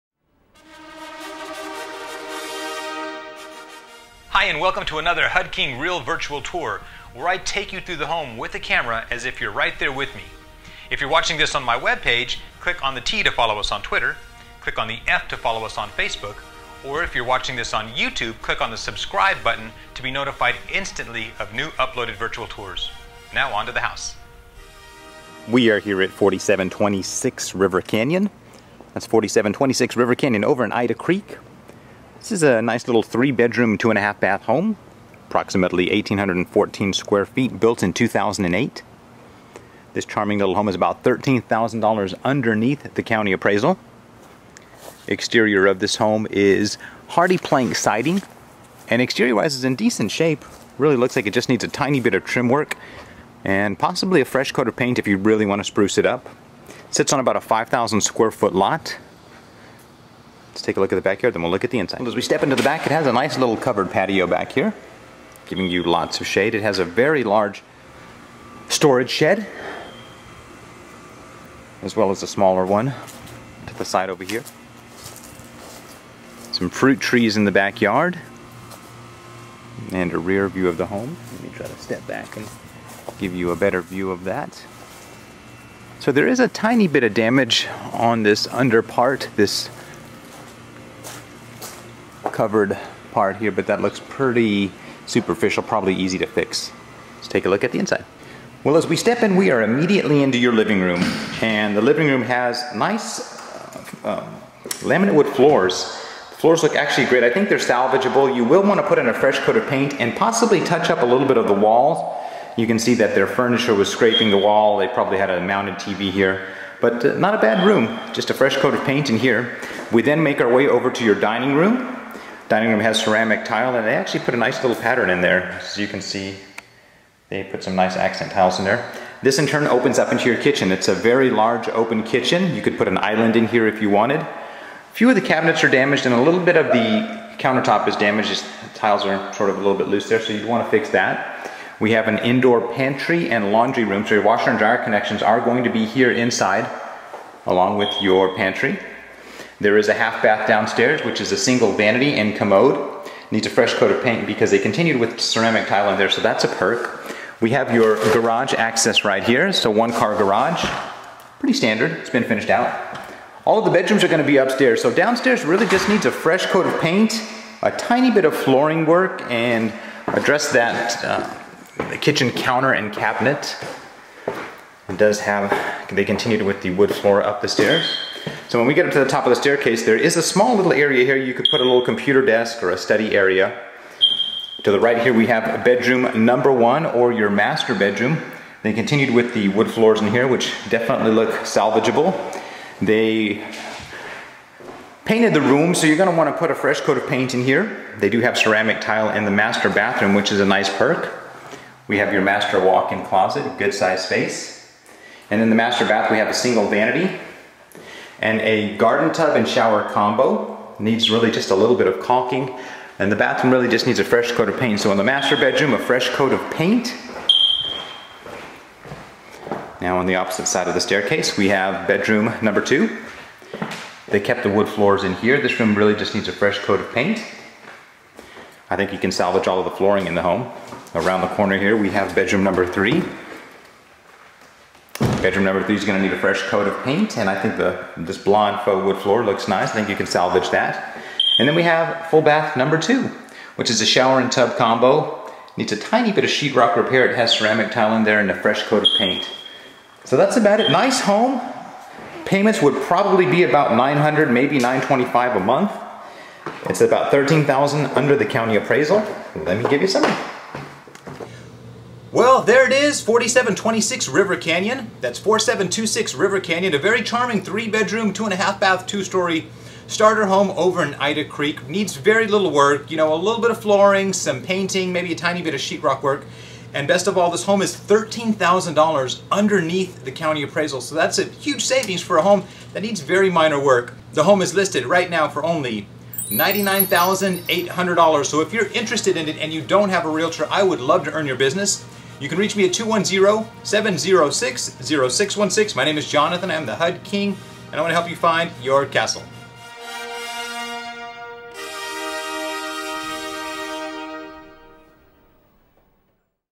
San Antonio HUD Homes -- HUD King tours 4726 River Canyon
Is San Antonio Real Estate a good investment? Watch as The HUD King delivers a real virtual tour of the HUD Home located at 4726 River Canyon, San Antonio, TX, 78222. This is a nice 3 bedroom 2½ bath 1,814 square foot home built in 2008. It needs the basic TLC and is listed for $99,800, plus HUD will pay 3% of your closing costs.